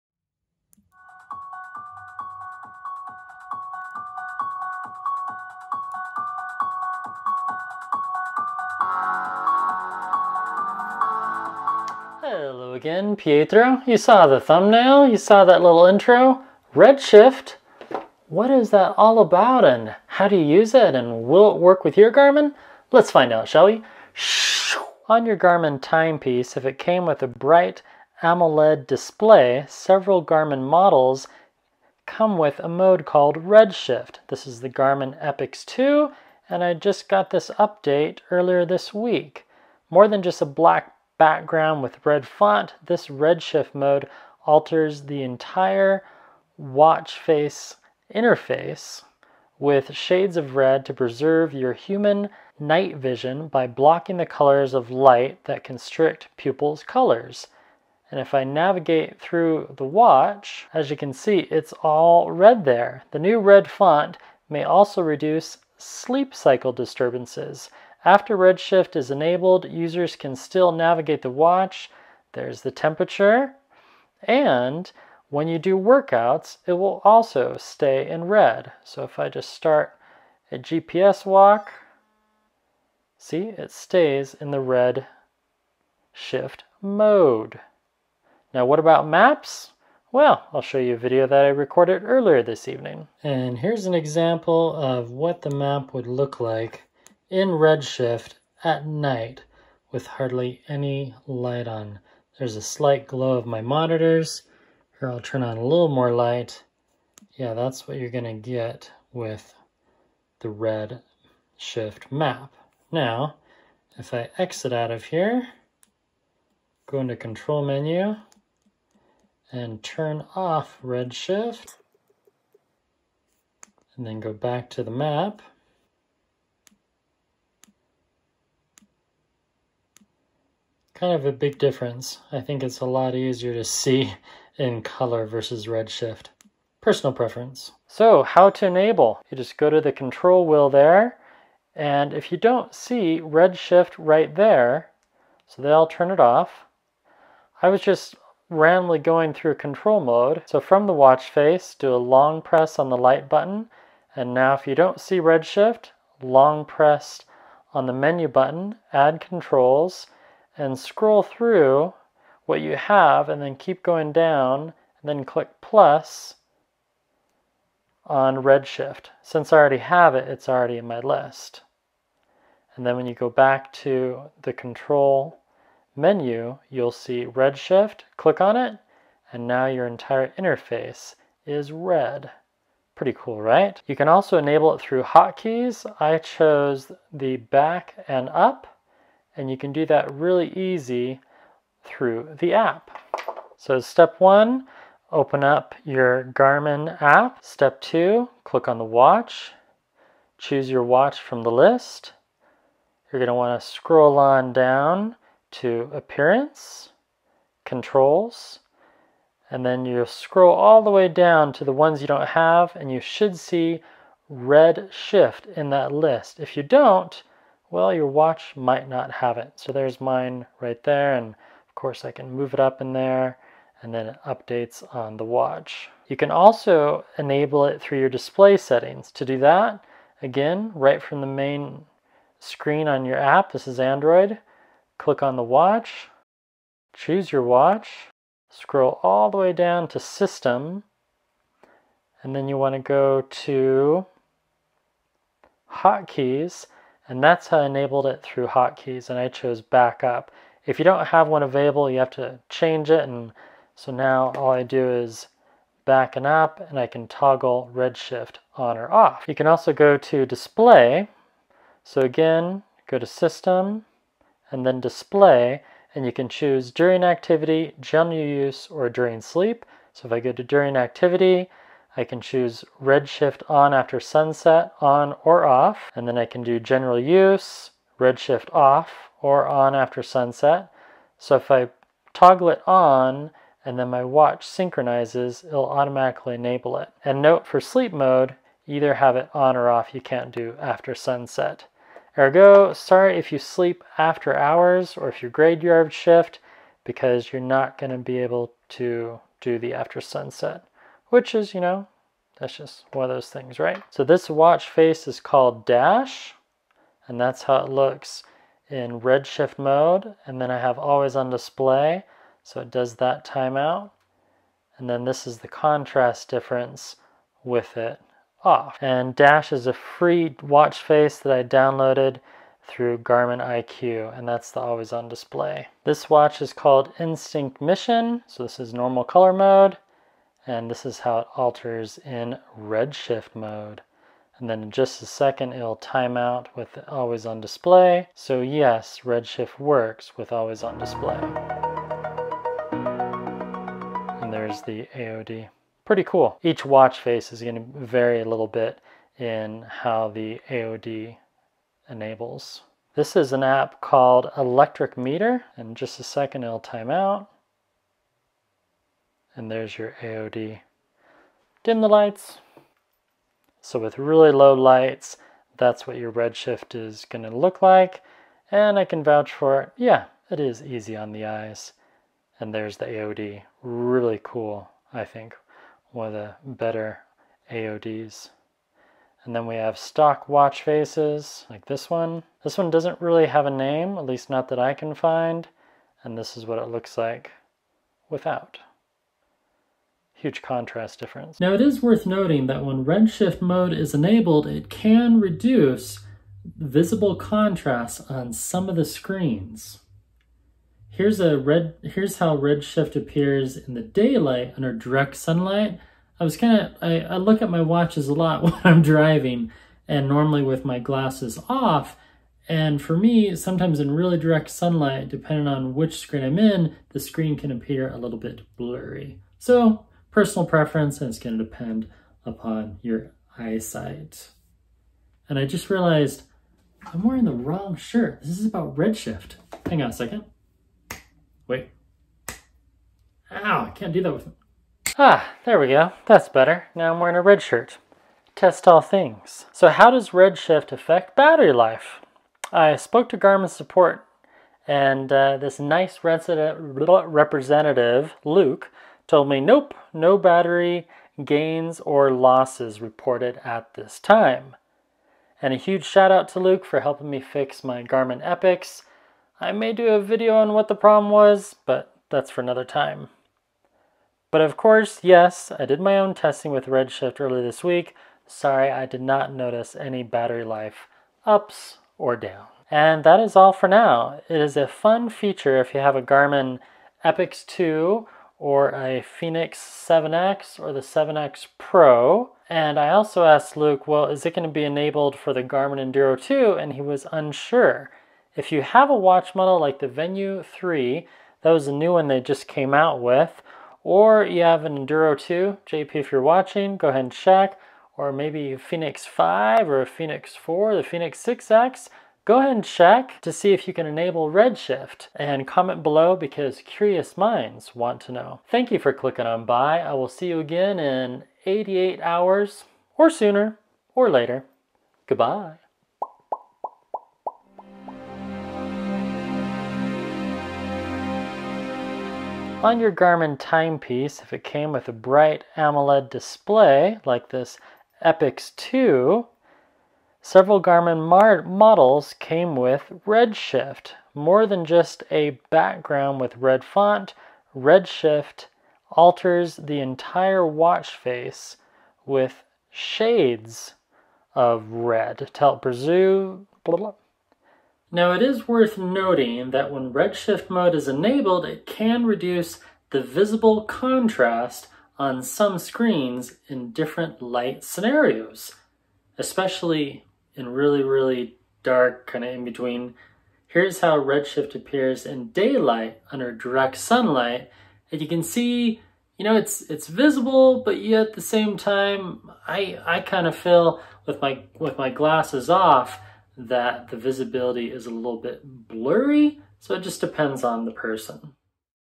Red Shift Mode...
Are missing the color red? Do you have a Garmin EPIX 2 or a Fenix 7X? Have you heard of Red Shift Mode? Yes, it is pretty cool and it is free with many AMOLED watches by Garmin...

I'm gifting you $35 off on Ultrahuman Ring; use my personal invite to claim your discount